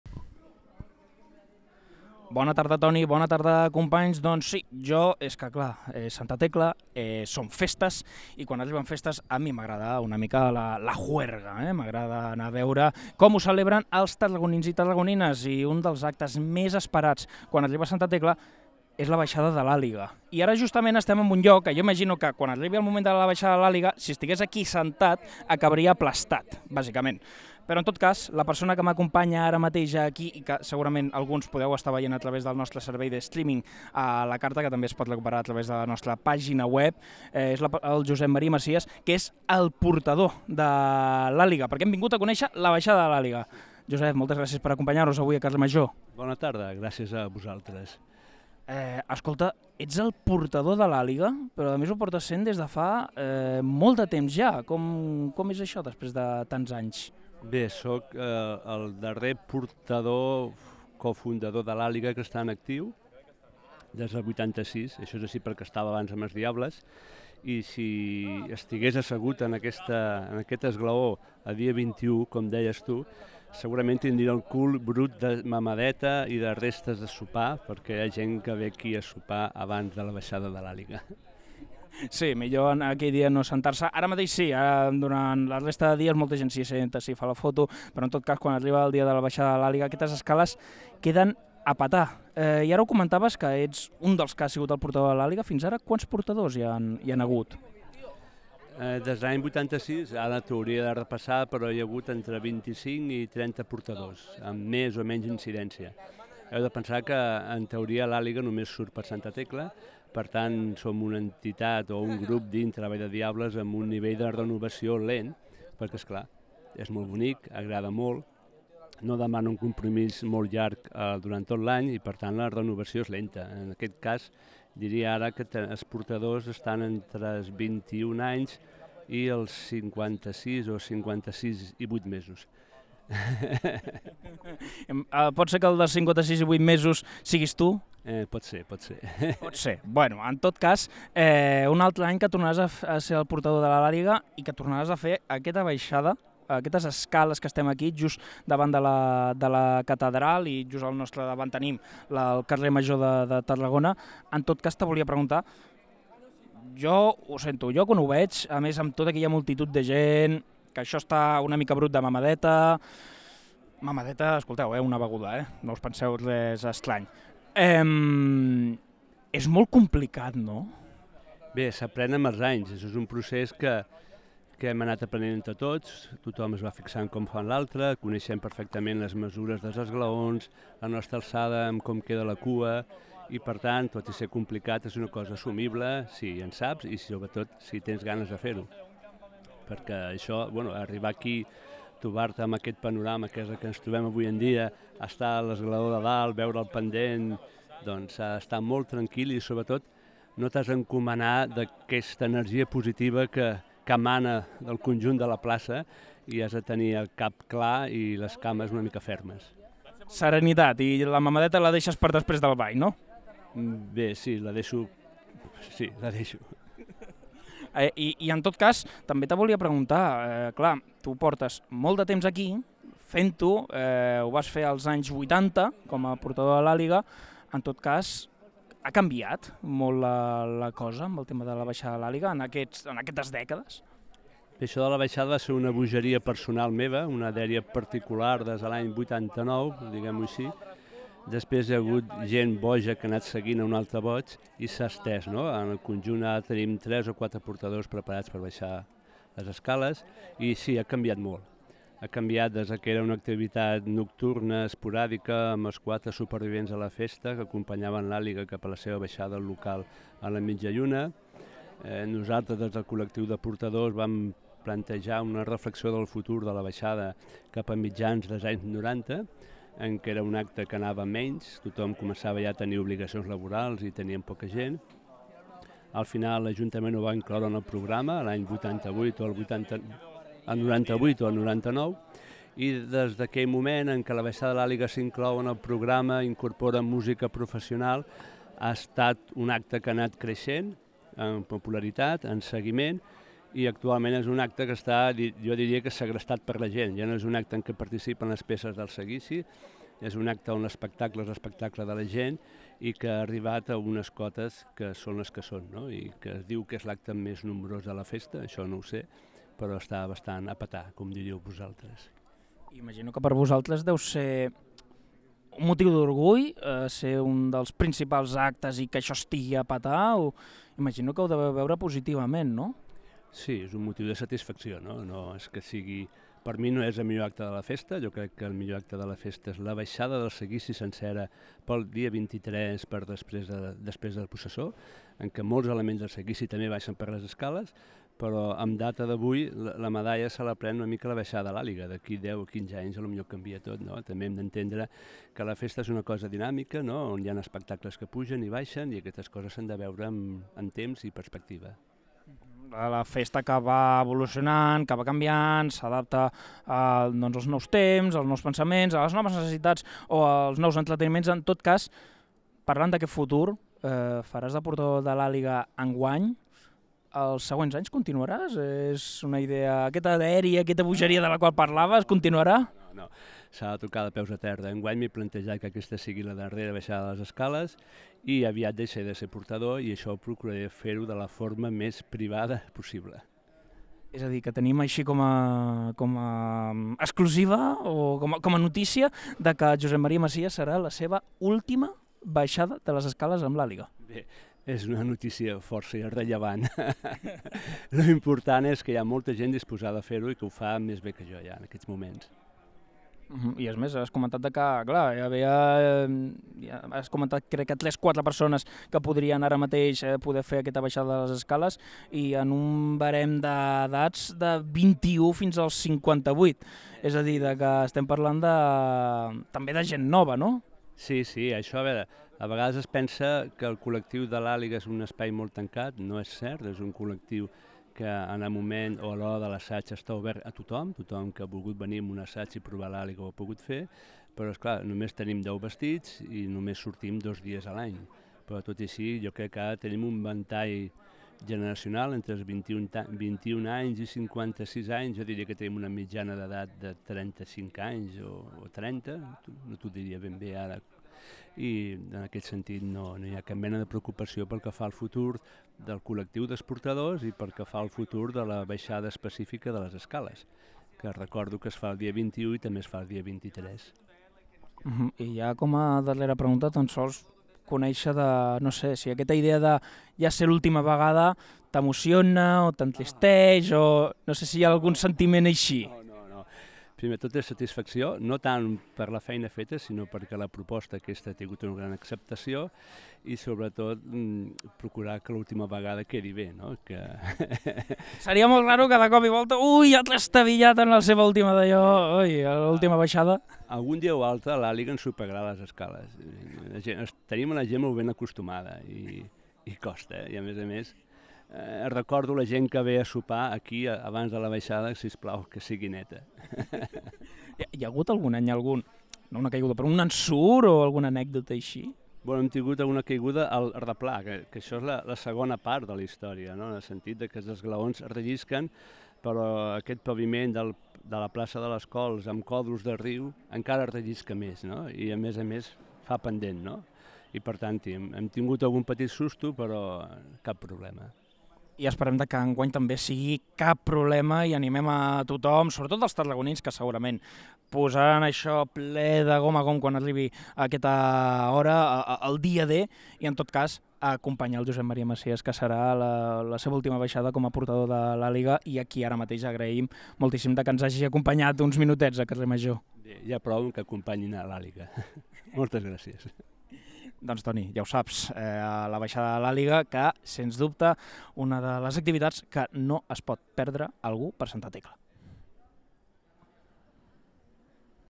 Baixada de l'Àliga de Tarragona
Aquest dijous LaFurgo de CarrerMajor es desplaça fins a Tarragona per conèixer com serà un dels actes més multitudinaris de les festes de Santa Tecla, la Baixada de l'Àliga. Per això parlem amb el portador de l'Àliga, Josep Maria Macías.